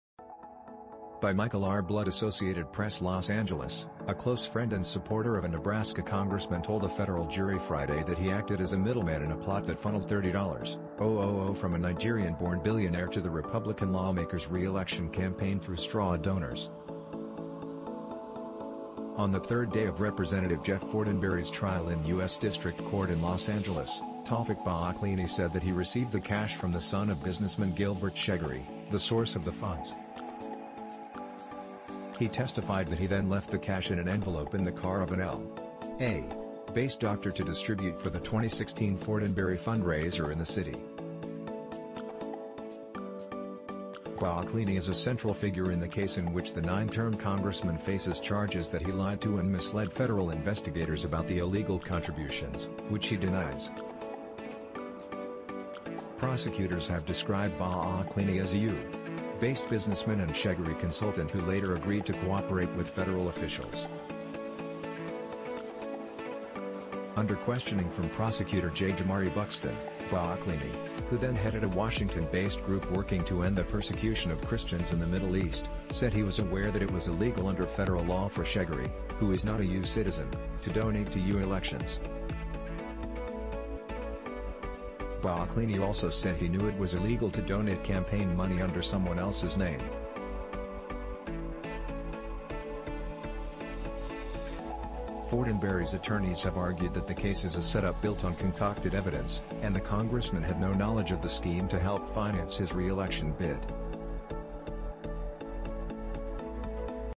Illegal $30K donation plot outlined at congressman’s trial
By Michael R. Blood | Associated PressLOS ANGELES — A close friend and supporter of a Nebraska congressman told a federal jury Friday that he acted as a middleman in a plot that funneled $30, 000 from a Nigerian-born billionaire to the Republican lawmaker’s reelection campaign through straw donors. On the third day of Rep. Jeff Fortenberry’s trial in U. S. District Court in Los Angeles, Toufic Baaklini said that he received the cash from the son of businessman Gilbert Chagoury, the source of the funds. He testified that he then left the cash in an envelope in the car of an L. A. -based doctor to distribute for the 2016 Fortenberry fundraiser in the city. Baaklini is a central figure in the case in which the nine-term congressman faces charges that he lied to and misled federal investigators about the illegal contributions, which he denies. Prosecutors have described Baaklini as a U. -based businessman and Chagoury consultant who later agreed to cooperate with federal officials. Under questioning from prosecutor J. Jamari Buxton, Baaklini, who then headed a Washington-based group working to end the persecution of Christians in the Middle East, said he was aware that it was illegal under federal law for Chagoury, who is not a U. citizen, to donate to U. elections. Baaklini also said he knew it was illegal to donate campaign money under someone else’s name. Fortenberry’s attorneys have argued that the case is a setup built on concocted evidence, and the congressman had no knowledge of the scheme to help finance his reelection bid. Baaklini described himself as a close friend of Fortenberry, and said the two bonded over what prosecutors have described as “the cause” — helping Christians and other persecuted minorities in the Middle East. The lead figures in the alleged plot all had ties to Baaklini’s group, In Defense of Christians, and are of Lebanese descent. Buxton used a series of texts to highlight the close relationship Baaklini shared with Fortenberry, including other fundraising appeals from the congressman. In other testimony, defense attorney Glen Summers continued his attempts to challenge the credibility of FBI special agent Todd Carter, a key player in the investigation. Carter has said he was suspicious of Fortenberry’s role — and a possible quid pro quo — after the congressman proposed a resolution condemning the persecution of Christians in the Middle East shortly after the February 2016 fundraiser. But Summers pointed out that the resolution was introduced in 2015, months before the event. Summers also noted that Fortenberry had nearly $1. 5 million in his political treasury at the end of March 2016, apparently suggesting he had no need to engage in a plot to gain $30, 000 in illegal funds. He questioned why the FBI never held a meeting to warn the congressman of a potential problem, instead of arriving unannounced at his home to question him.